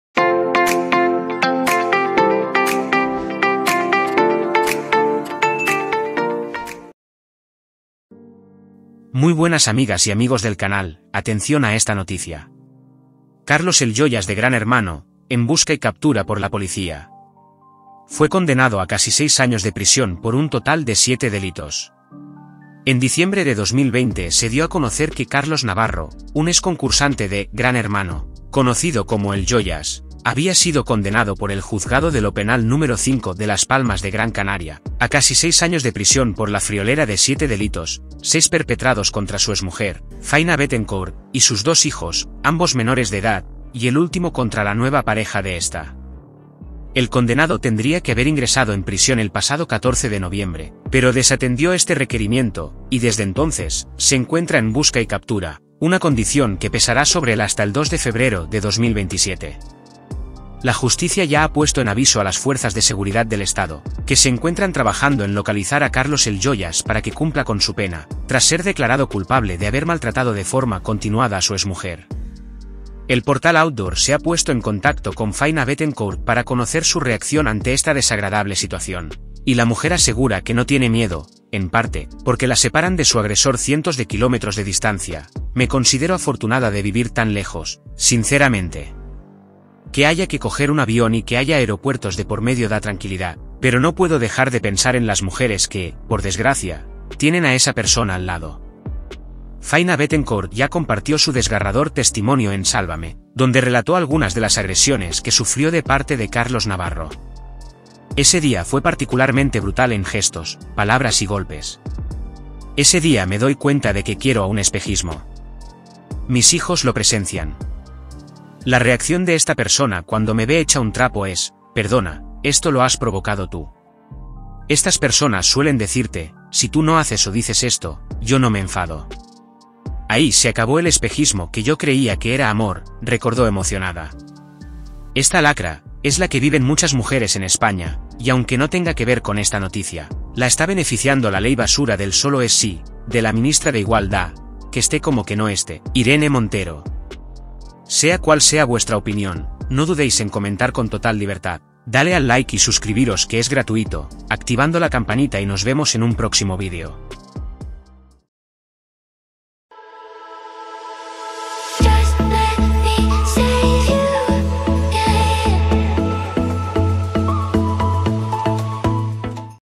🔴 Carlos EL YOYAS de GRAN HERMANO en BUSCA y CAPTURA por la POLICIA
En diciembre de 2020 se dio a conocer que Carlos Navarro, un exconcursante de “Gran Hermano” conocido como “El Yoyas”, había sido condenado por el Juzgado de lo Penal Número 5 de Las Palmas de Gran Canaria a casi seis años de prisión por la friolera de siete delitos, seis perpetrados contra su exmujer, Fayna Bethencourt, y sus dos hijos (ambos menores de edad), y el último contra la nueva pareja de esta.

El condenado tendría que haber ingresado en prisión el pasado 14 de noviembre, pero desatendió este requerimiento y, desde entonces, se encuentra en busca y captura, una condición que pesará sobre él hasta el 2 de febrero de 2027. La Justicia ya ha puesto en aviso a las Fuerzas de Seguridad del Estado, que se encuentran trabajando en localizar a Carlos “El Yoyas” para que cumpla con su pena, tras ser declarado culpable de haber maltratado de forma continuada a su exmujer.

El portal “Outdoor” se ha puesto en contacto con Fayna Bethencourt para conocer su reacción ante esta desagradable situación, y la mujer asegura que no tiene miedo, en parte, porque la separan de su agresor cientos de kilómetros de distancia: “Me considero afortunada de vivir tan lejos, sinceramente. Que haya que coger un avión y que haya aeropuertos de por medio da tranquilidad, pero no puedo dejar de pensar en las mujeres que, por desgracia, tienen a esa persona al lado”.

Fayna Bethencourt ya compartió su desgarrador testimonio en “Sálvame”, donde relató algunas de las agresiones que sufrió de parte de Carlos Navarro. “Ese día fue particularmente brutal en gestos, palabras y golpes. Ese día me doy cuenta de que quiero a un espejismo. Mis hijos lo presencian. La reacción de esta persona cuando me ve hecha un trapo es: ‘Perdona, esto lo has provocado tú’. Estas personas suelen decirte: si tú no haces o dices esto, yo no me enfado. Ahí se acabó el espejismo que yo creía que era amor”, recordó emocionada.